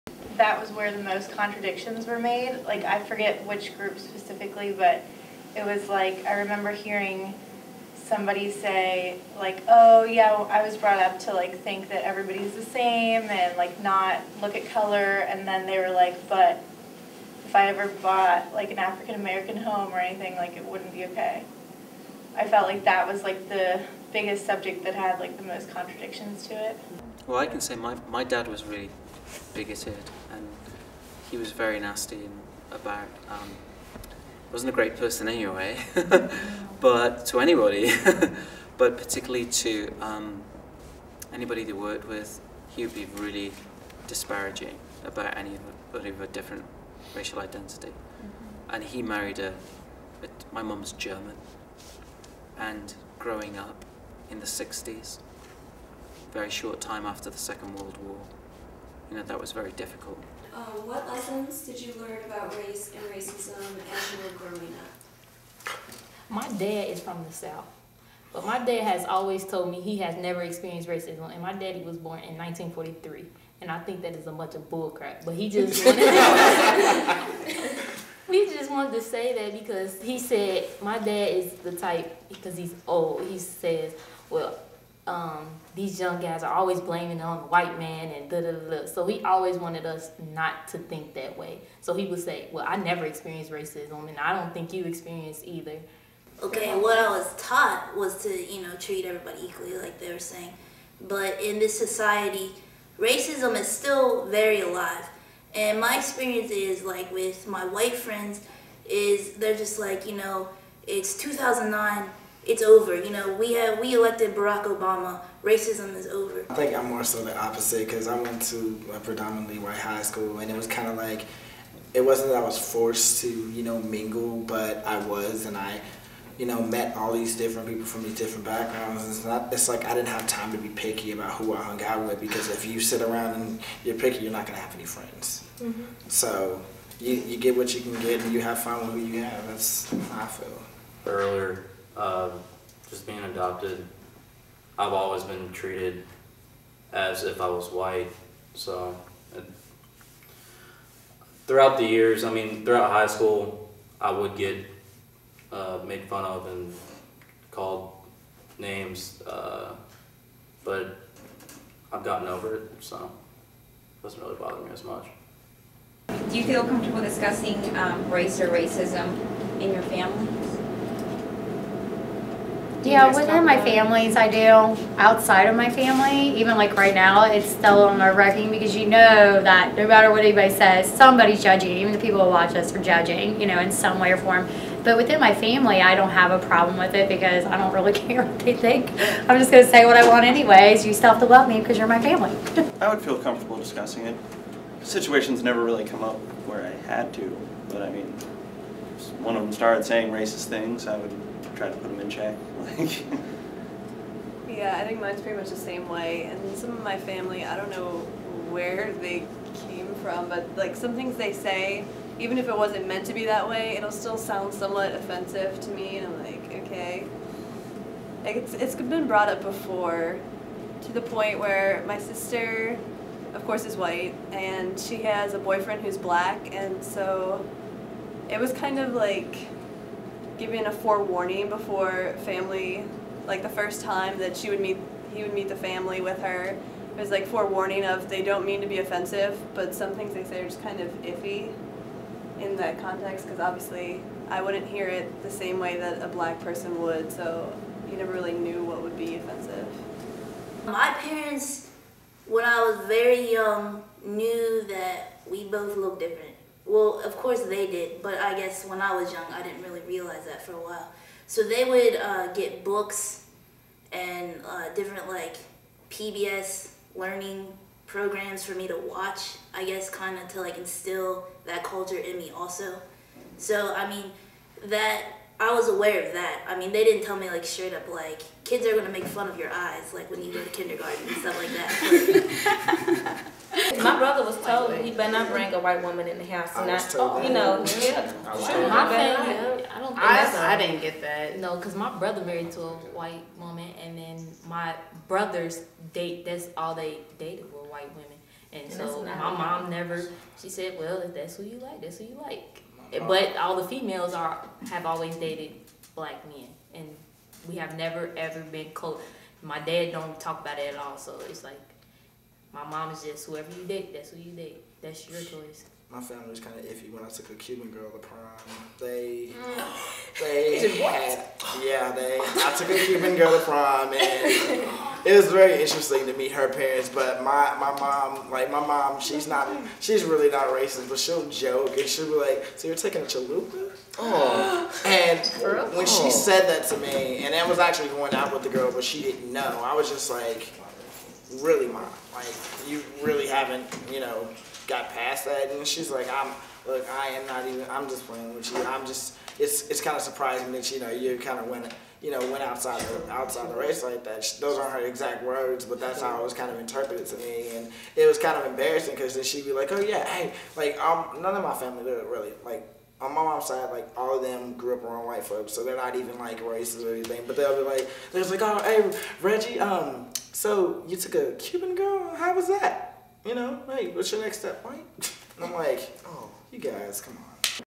Through Their Eyes pt2 - Racism Growing Up
A Documentary from the perspective of NKU college students. Four ethnic groups were interviewed on campus to discuss their views on racial issues. The interviews were conducted by the Interracial Communications class of the Spring of '09.  The following reflects the views of those individuals.